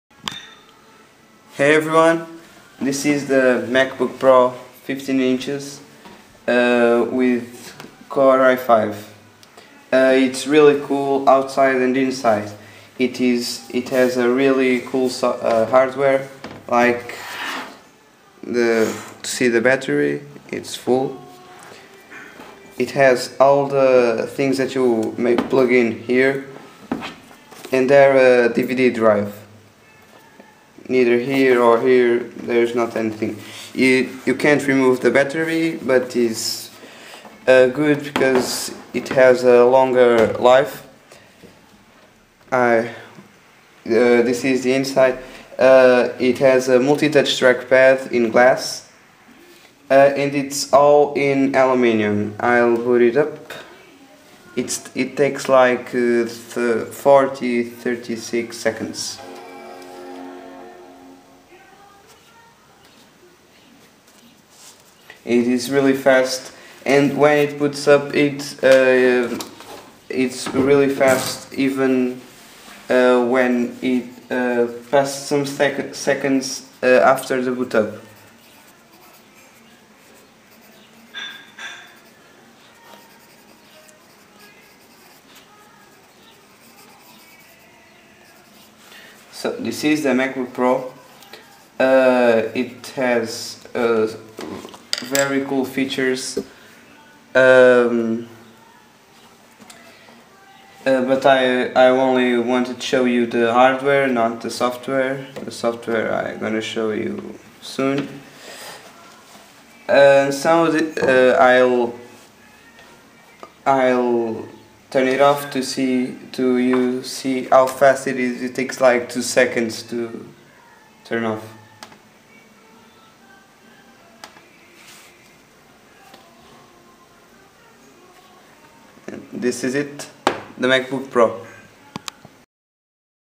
Macbook Pro 15'' intel core i5
I'm showing you the hardware of a macbook pro with 15'' and intel core i5, enjoy the video